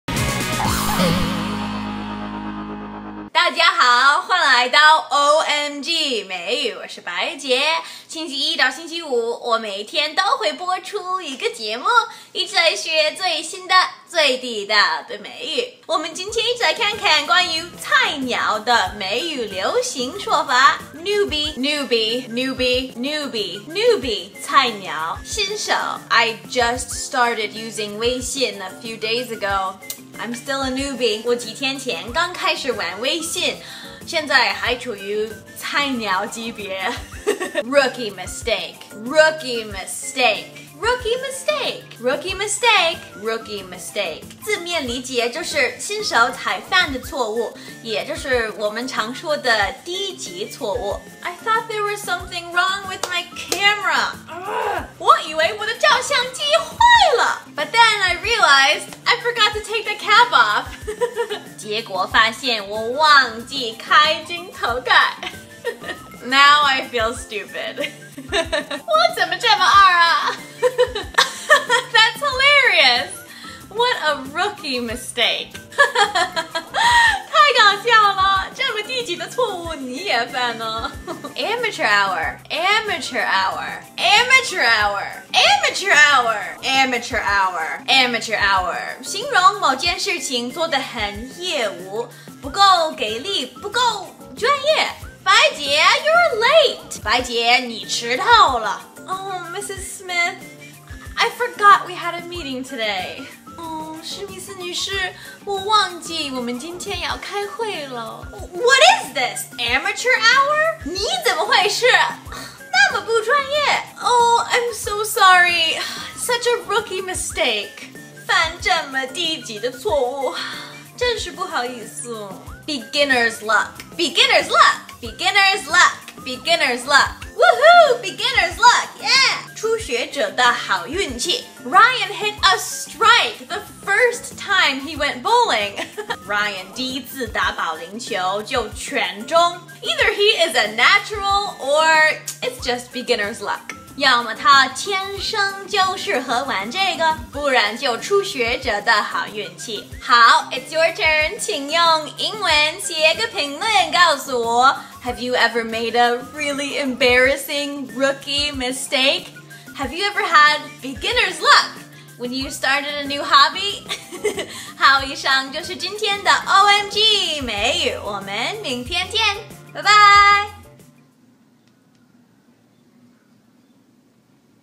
OMG!美语 Rookie Mistake!
今天白洁和大家一起来学关于"菜鸟"的美语流行说法。

newbie - 新手，菜鸟

I just started using Weixin a few days ago. I'm still a newbie.

我几天前刚开始玩微信，现在还处于菜鸟级别。

rookie mistake - 字面理解就是新手才犯的错误，也就是我们常说的低级错误

A: I thought there was something wrong with my camera. 我以为我的照相机坏了。 But then I realized I forgot to take the lens cap off. 结果发现是我忘记开镜头盖。Now I feel stupid. 我怎么那么二啊。

B: hahaha That's hilarious. What a rookie mistake!

哈哈哈。太搞笑了。 这么低级的错误你也犯！

amateur hour - 形容某件事情做的很业余，不够专业，不够给力。

A: Baijie! You are late!

白洁，你迟到了！

B: Oh Ms. Smith, I forgot we had a meeting today.

哦史密斯女士，我忘记今天要开会了。

A: What is this, amateur hour?!

你怎么回事？那么不专业！

B: I'm so sorry for making such a rookie mistake!

犯这么低级的错误, 真是不好意思。

beginner's luck - 初学者的好运气

Ryan hit a strike the first time he went bowling. Either he's a natural, or it's just beginner's luck.

Ryan第一次打保龄球就全中! 要么他天生就适合玩这个，不然就是初学者的好运气。